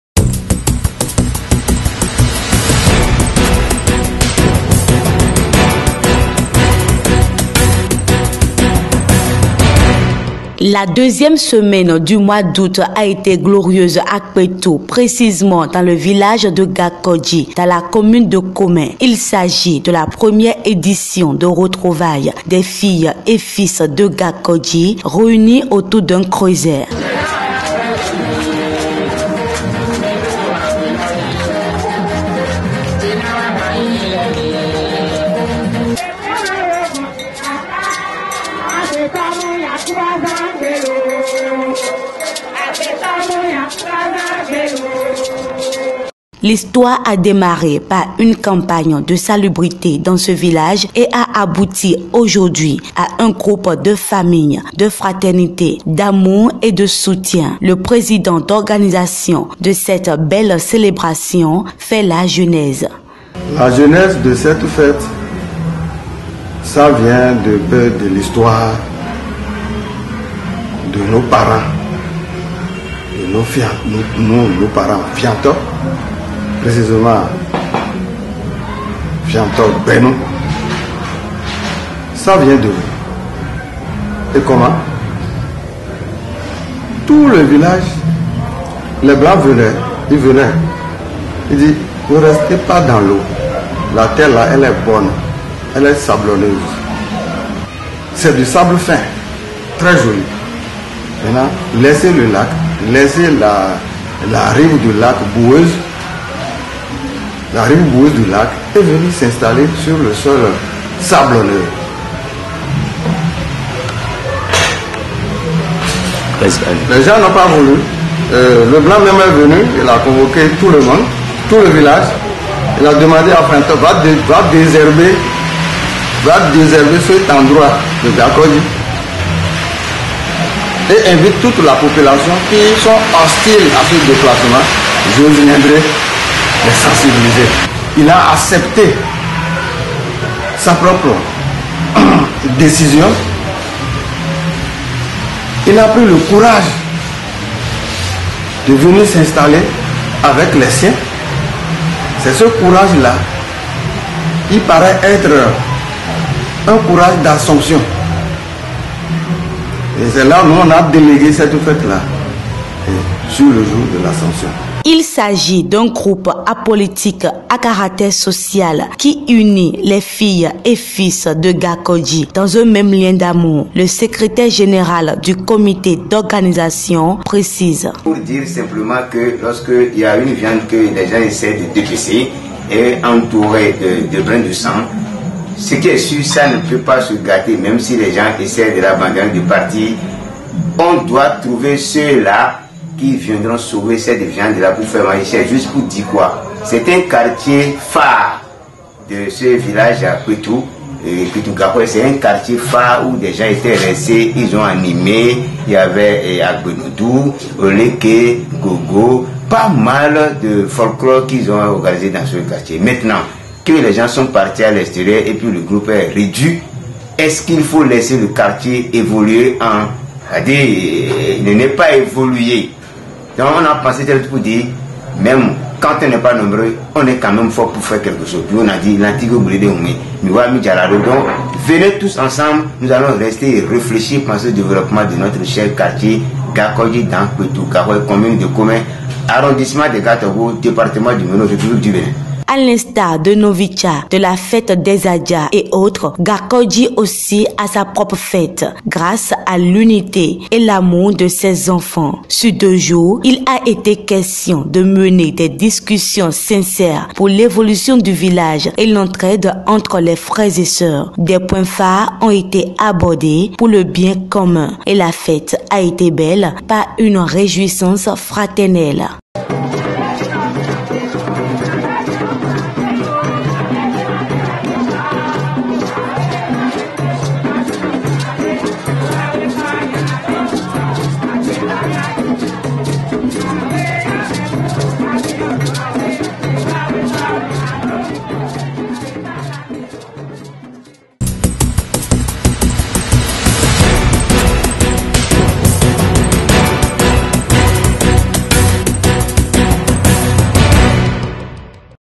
Fête de retrouvailles NONVI-EVAMI ( Gacodji 2022 )
Les filles et fils de Gacodji dans la commune de Comè se sont retrouvés pour célébrer leur fête. Il s'agit de NONVI-EVAMI qui est à sa première édition cette année 2022 le mois d'août dernier. A l'ordre du jour plusieurs activités sociales  et festives à la hauteur de cet événement dans la vie de Gacodji. La fête était belle et riche en couleurs dans la fraternité et la convivialité. Une équipe de AFRIK CHRONO TV était de la partie. Voici son reportage.
AFRIK CHRONO TV la course à l'info réelle.